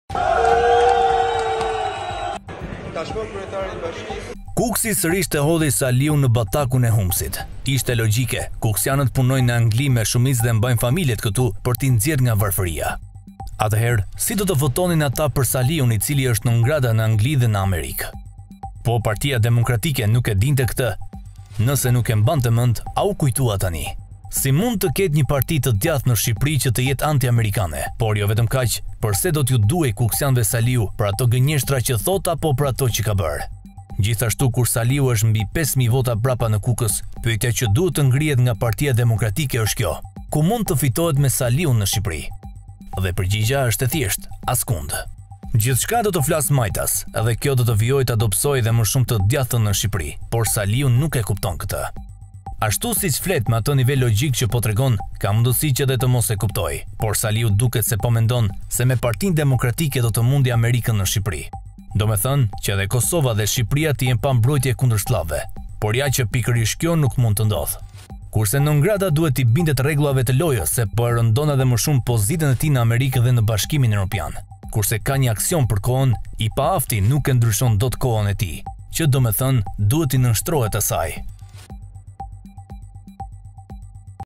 Spartak Ngjela: Ku mund të fitohet sot me Saliun në Shqipëri?Jepni mendimin tuaj..
Spartak Ngjela: Ku mund të fitohet sot me Saliun në Shqipëri?
Jepni mendimin tuaj..